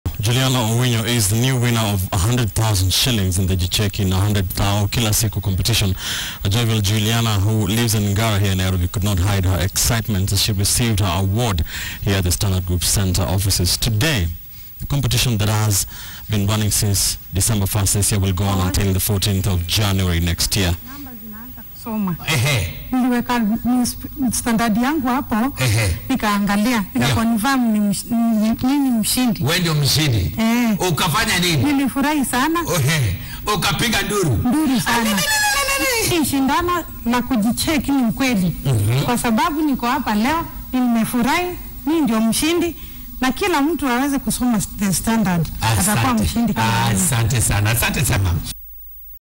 Julianna Owinyo excited as she received Ksh 100,000 in the ‘jicheki na 100 thao’ competition
Julianna Owinyo is the  new winner of ksh 100,000 in the  ‘jicheki na 100 thao kila siku’  competition . Jovial Julianna Owinyo who lives in Ngara could not hide her excitement as she received her award at the standard group offices today. The competition that has been running since December 1st this year  will go on until 14 th  January next year and will see more lucky winners walk away with a cash prize of 100,000 shillings for buying the standard newspaper.